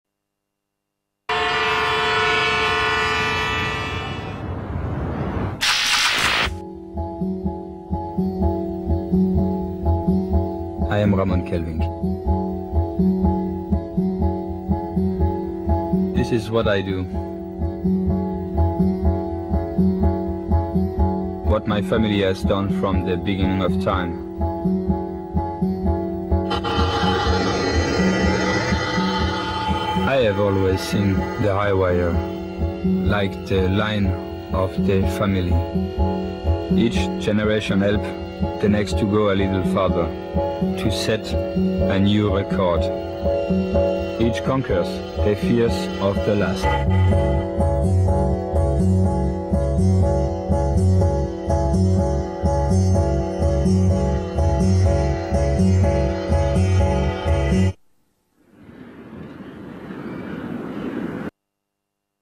Embassy Films Croatia - Nikon
Nikon Commercial shot in Dubrovnik Croatia, production service  by Embassy Films
Director's cut